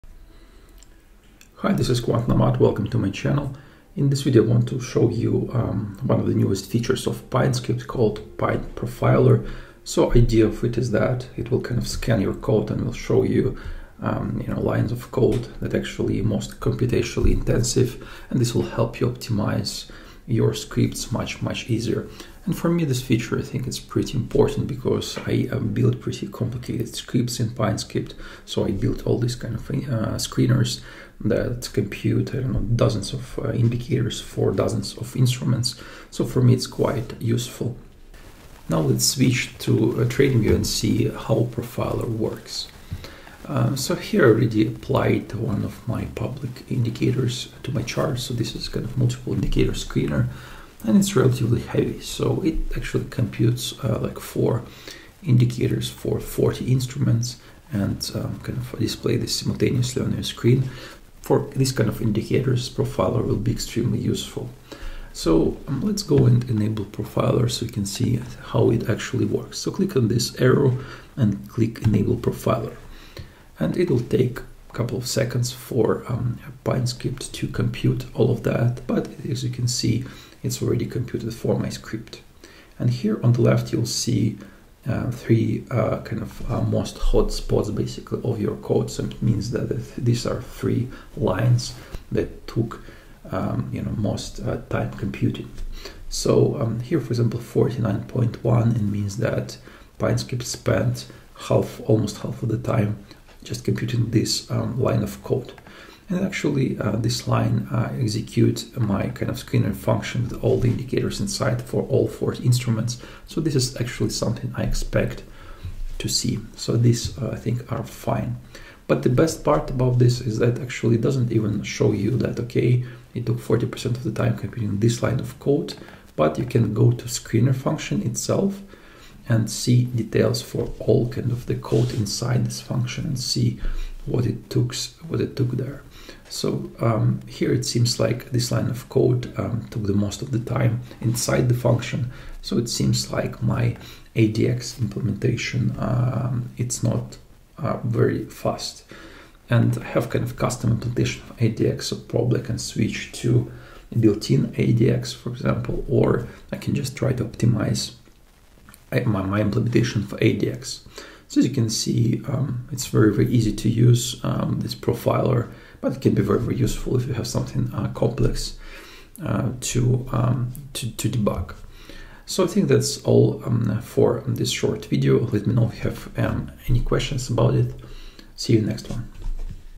TradingView Update: Pine Script Profiler - How To Use It?
Discover the latest feature on TradingView that’s set to help your trading strategies – the Pine Script Profiler! In this video, we dive into how this powerful tool can help you optimize your scripts, enhance performance, and improve your trading efficiency.